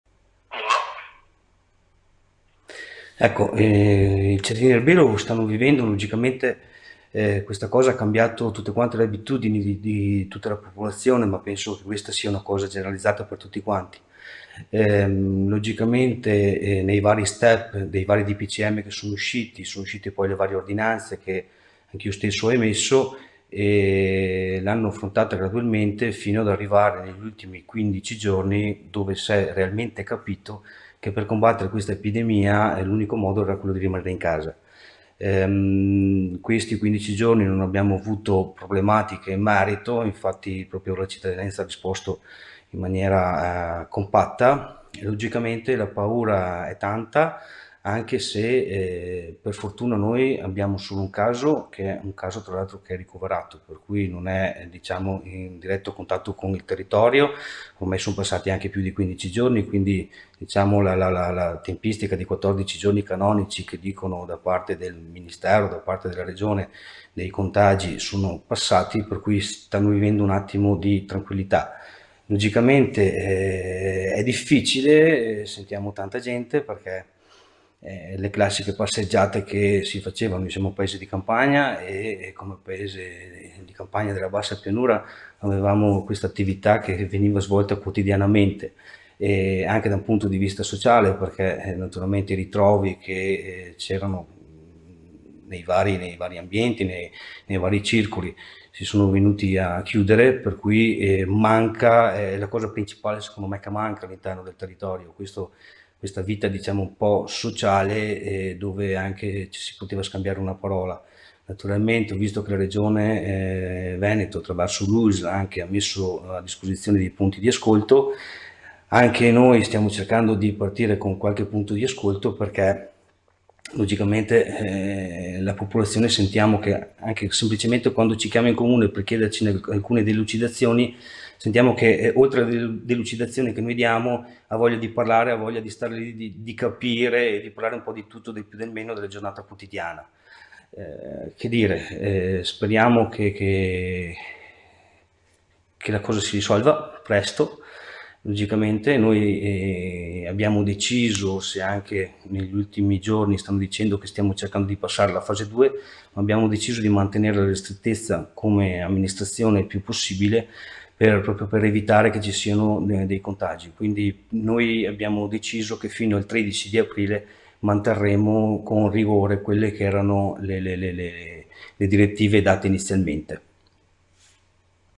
Speciale coronavirus: Nicola Martini, sindaco di Erbè
Speciale coronavirus: Nicola Martini, sindaco di Erbè. La situazione nel comune, durante l'emergenza Covid-19.